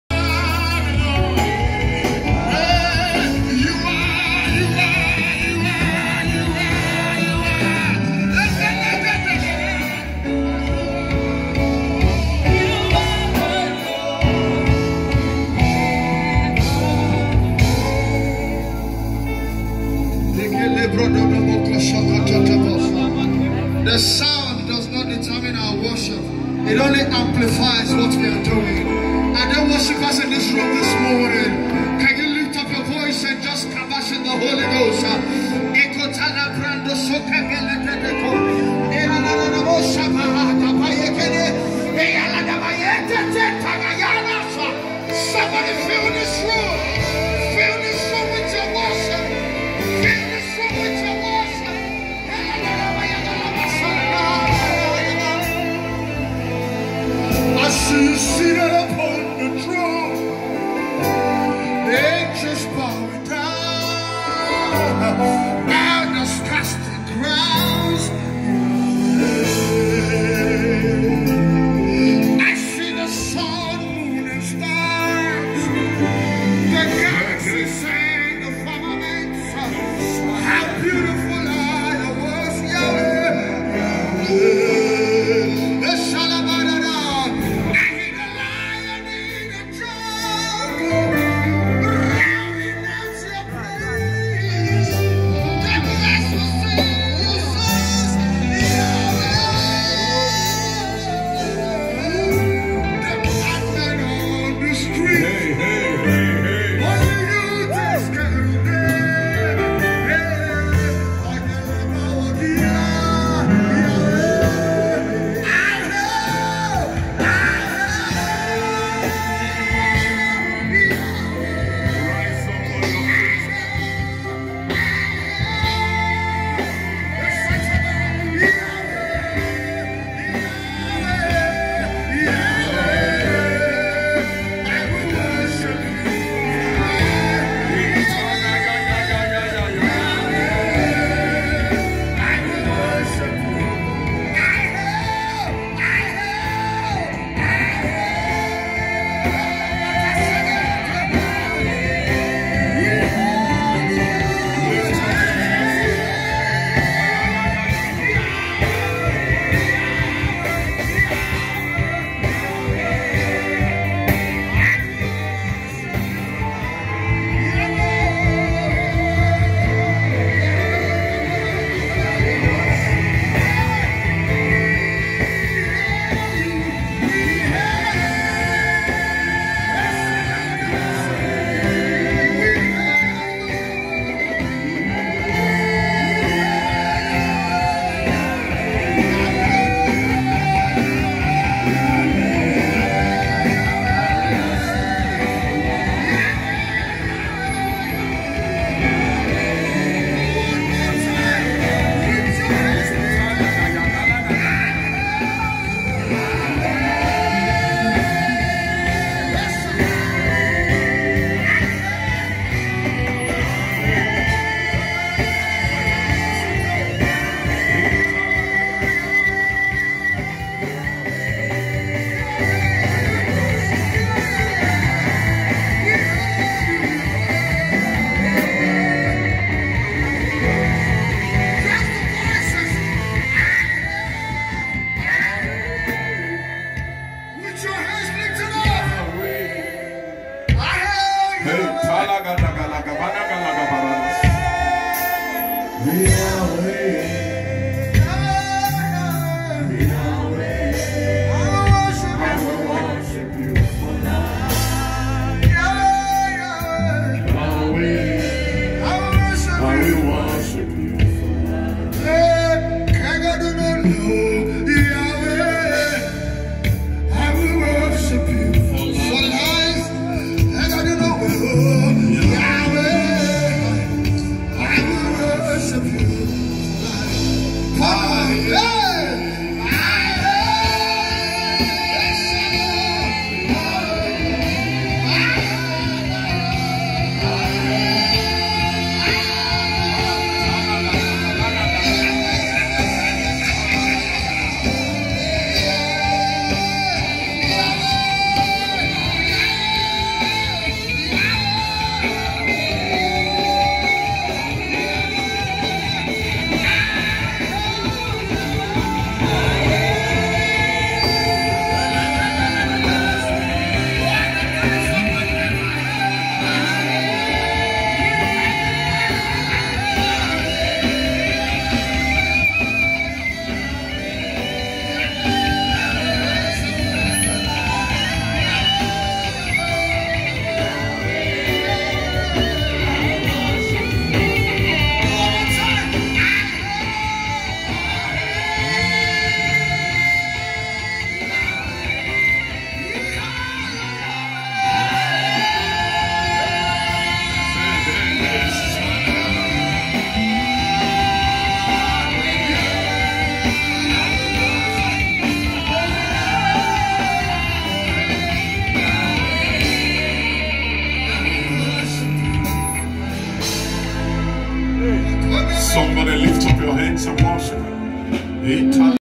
What An Electrifying Moment Of Worship In Church Today? The Presence Of God Was Heavy.
What an amazing time in God's presence today in service. Choir ministration was top notch and the presence of God was heavy like never before.

Please watch and drop your comments.